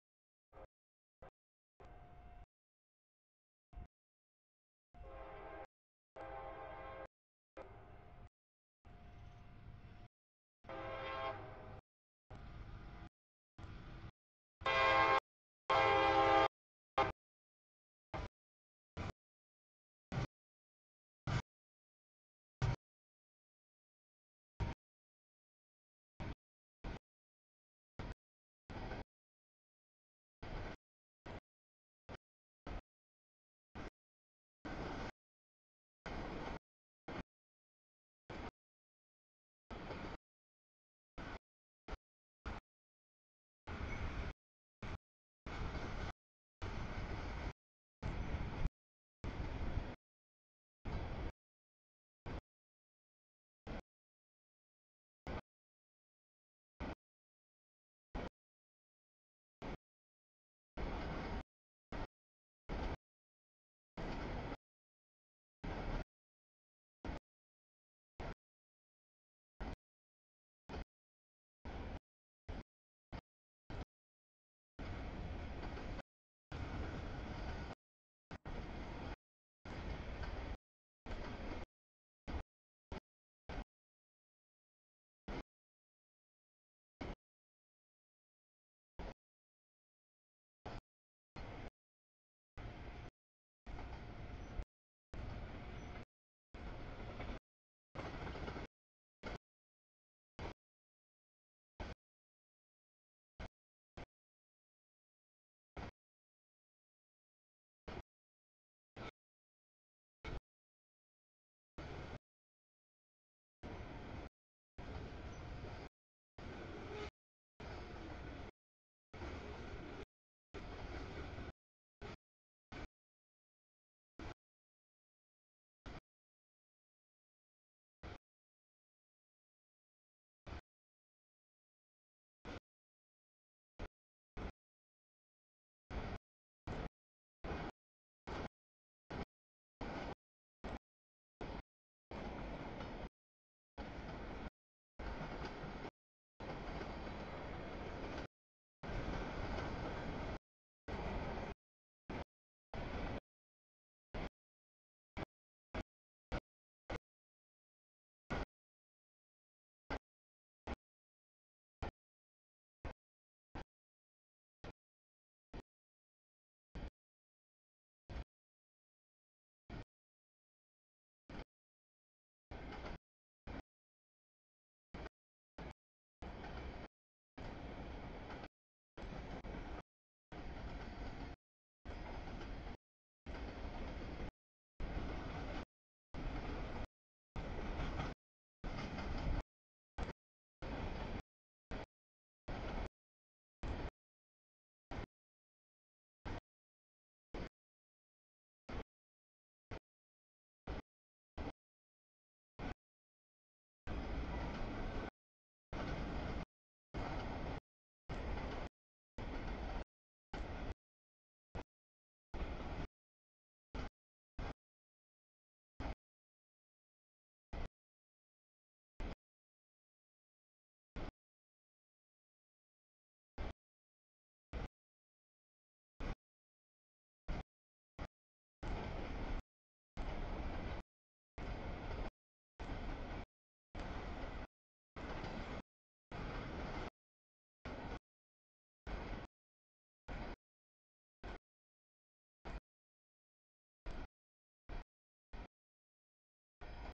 5- 18-2019 13:44 nb mix, epic trash CSX 4017, 5243 Q434
00:13 sb stack CSX 775 Q162
00:56 sb mix CSX 3381 Q409
01:10 nb stack trailers CSX 3136 Q009
01:27 nb stack CSX Q159
02:16 sb stack CSX 5274 Q160
02:41 nb stack CSX Q157
06:04 sb auto rack CSX 992 Q261
06:56 sb stack trailers CSX 915, 3391 Q010
07:12 sb stack trailers CSX 3029, 3054 Q158
07:19 nb stack trailers CSX 3250, 970 Q003
07:34 nb stack trailers CSX 799, 754 Q007
07:48 nb auto rack CSX 3202 Q253
08:45 sb tanks CSX 5377, 153 K678
11:36 sb stack CSX 439, 5299 Q158
12:44 sb green trash, mix CSX 3343 DPU 3111 Q439
13:09 nb mix, nysw cut, orange trash 3443, 3339 DPU 819, 3423 Q410
13:44 nb mix, epic trash CSX 4017, 5243 Q434
14:00 nb mix, green trash CSX 3116 DPU 770 Q438
15:10 sb mix, blue trash CSX 7901, 214, LTEX 2092, 2093 Q433
15:50 sb auto rack CSX 916 DPU 3271 Q234